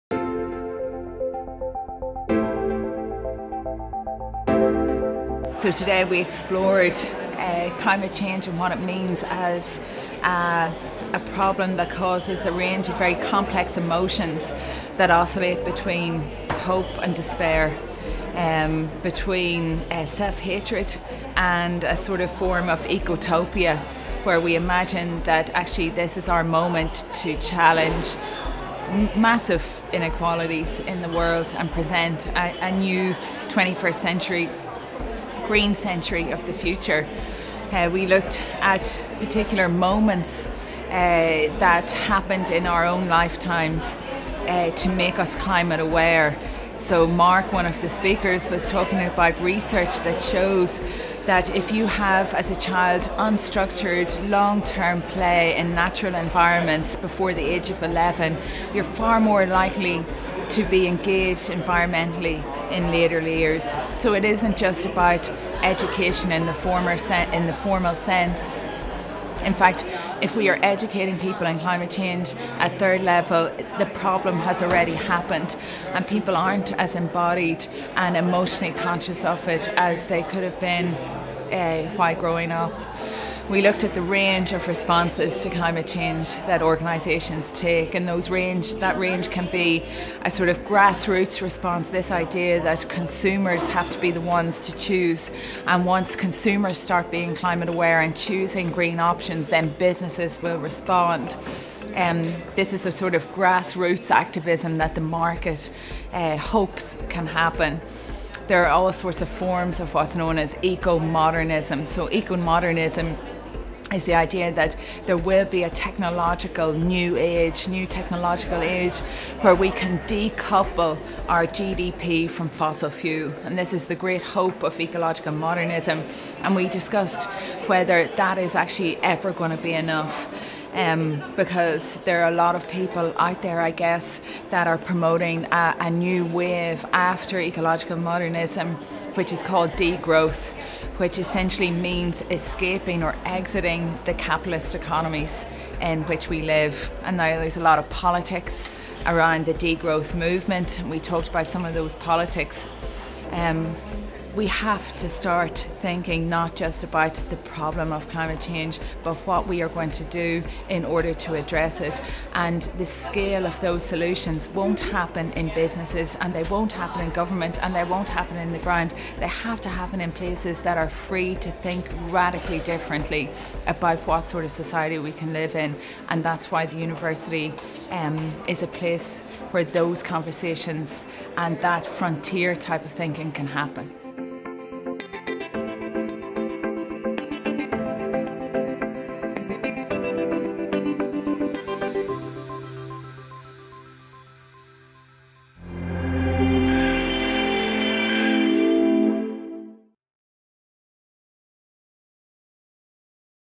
What Climate Science Wants Business to do - Dr. Norah Campbell
There is a high consensus within climate science that we have one decade until feedback and cumulative effects lead to a point of no return and the eventual collapse of the biosphere. While consensus converges on the scale and scope of the climate and environmental emergency, there is less on what is to be done. Norah Campbell discusses the eco-psychological, geo-physical and socio-economic scales of response with a writer, a researcher in sustainable consumption and a Corporate Sustainability Officer.